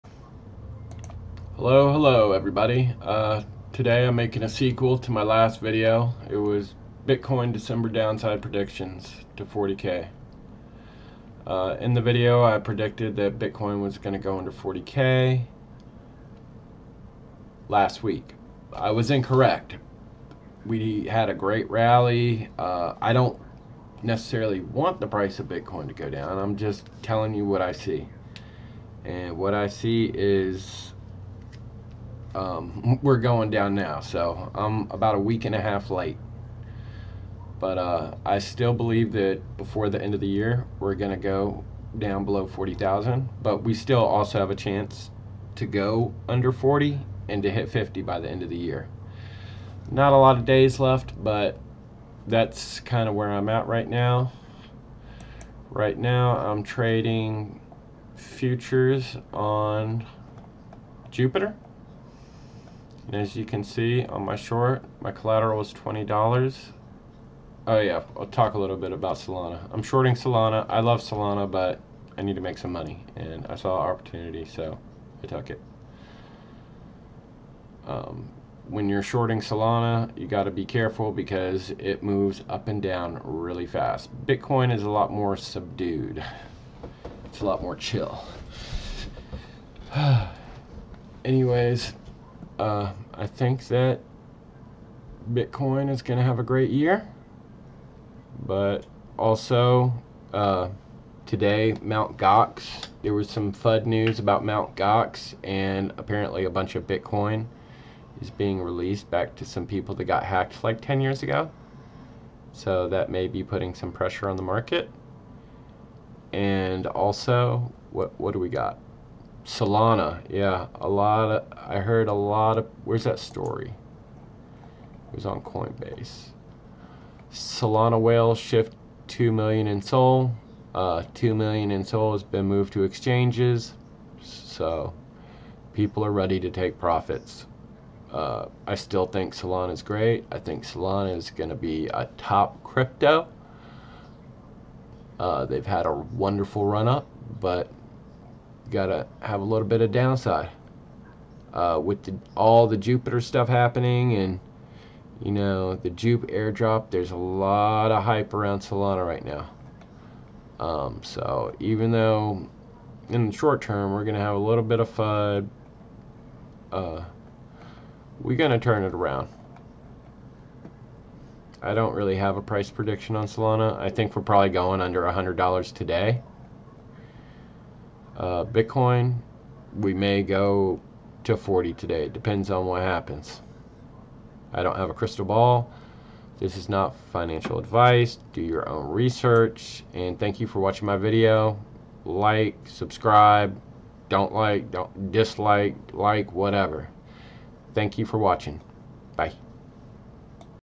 EOY Crypto predictions. NFA DYOR Solana Bitcoin BTC SOL
EOY crypto predictions and an apology. Last year I said it would be a merry HEXMAS and well it wasnt.